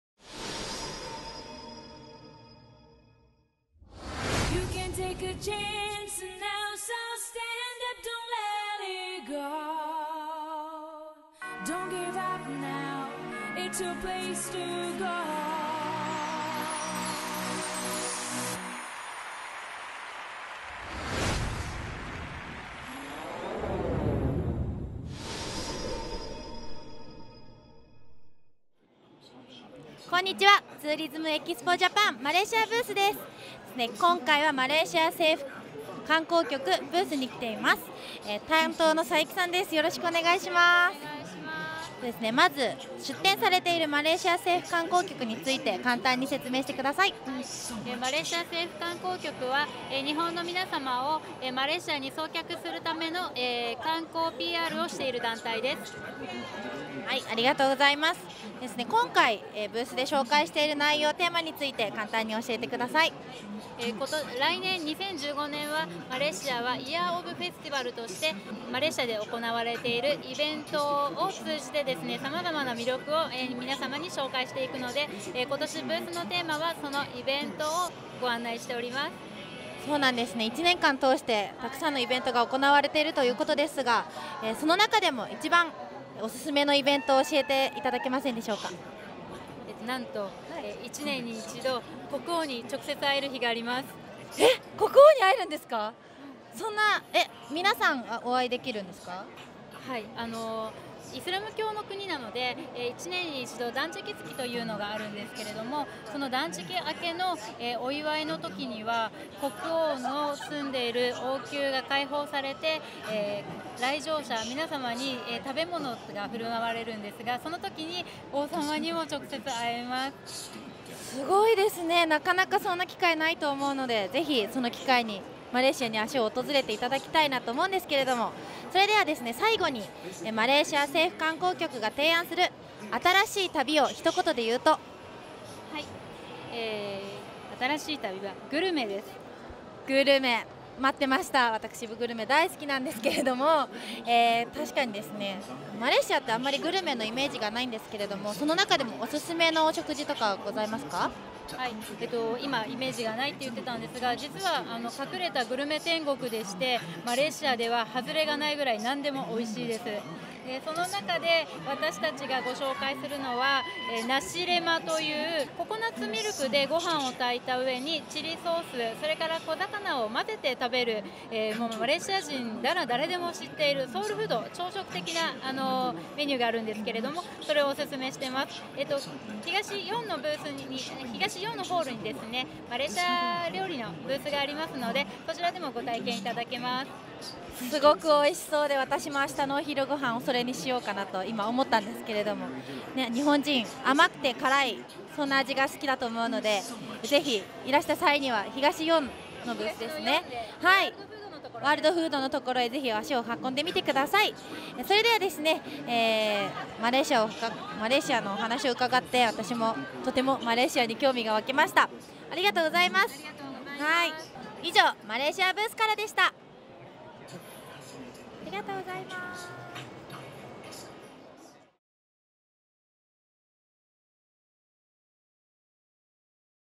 【ブースレポート】マレーシア観光局編
ツーリズムEXPOジャパン2014に出展されている、マレーシア観光局のブースに来ています。担当の佐伯さんに、今回のブースの特徴や見どころをはじめ、マレーシアの魅力をたっぷりと紹介してもらいました。